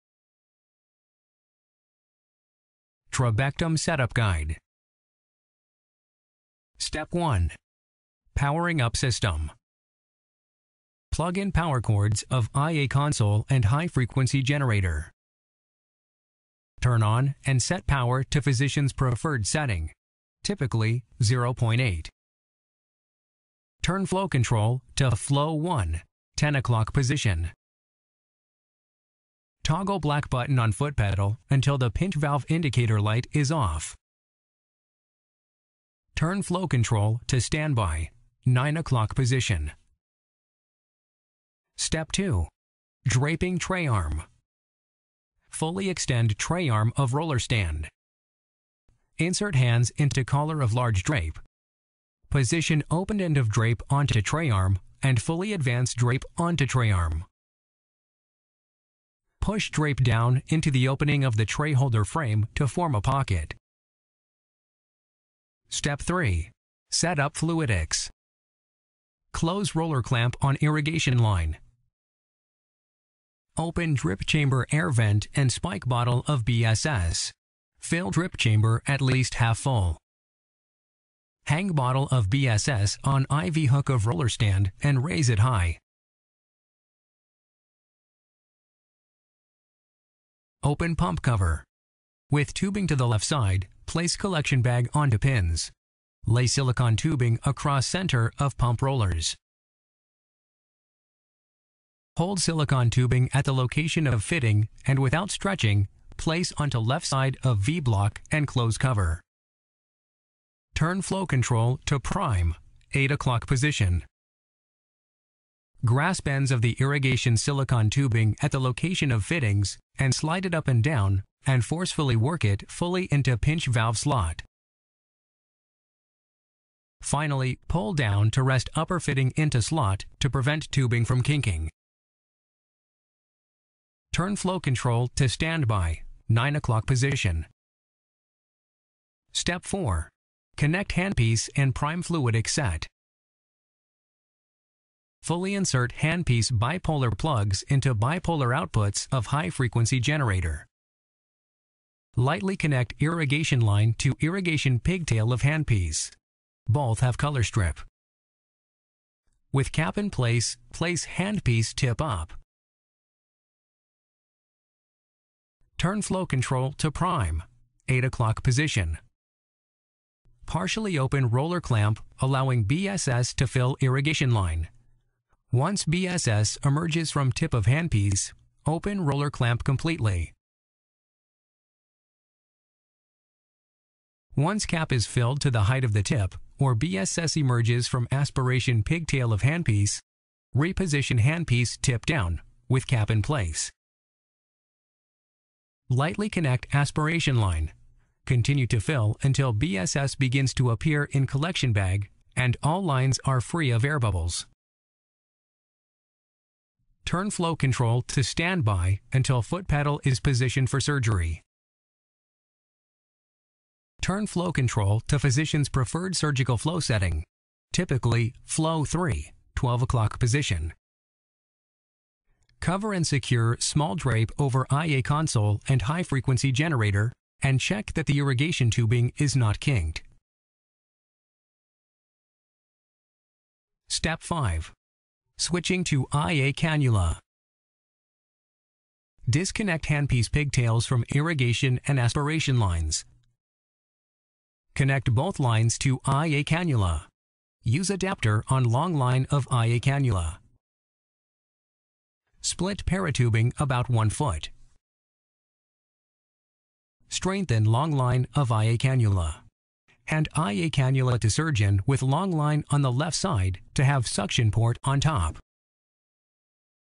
Trabectome Equipment Set-Up (4:27 min)
This tutorial takes you through the steps of setting up the Trabectome for surgery.